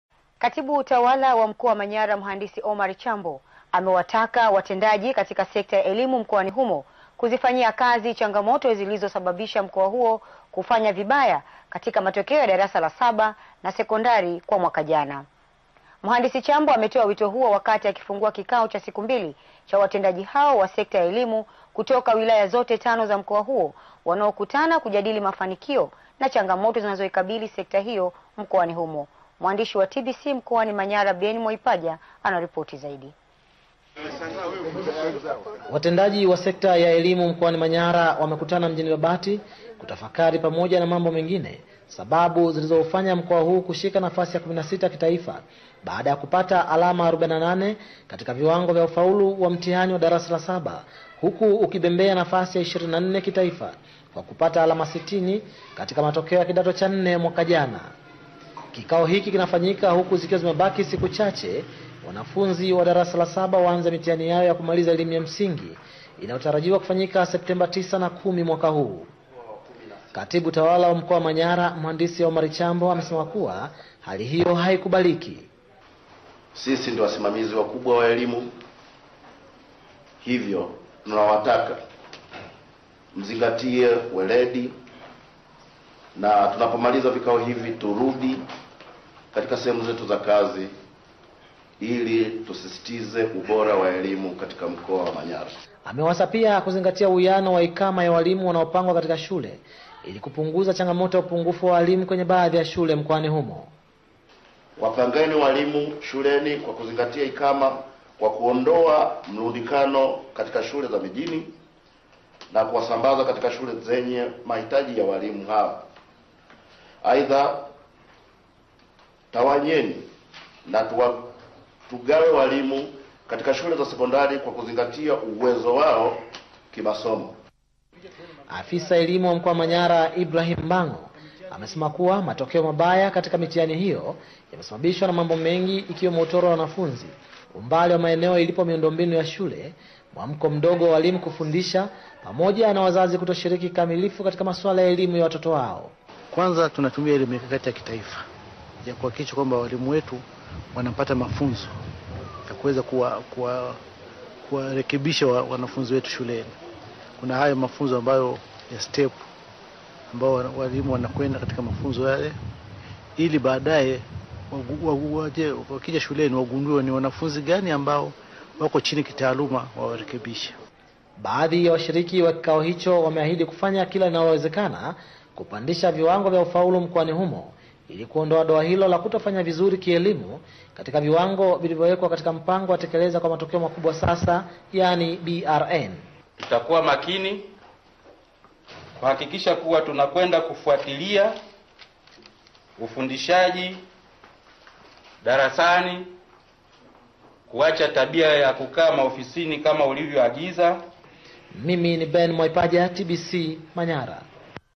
HABARI - AUGUST.18.2015 | TBC
watendaji wa sekta ya elimu Manyara wametakiwa kutafuta mbinu mbadala ili kuinua kiwango cha ufaulu kwa wanafunzi darasa la saba.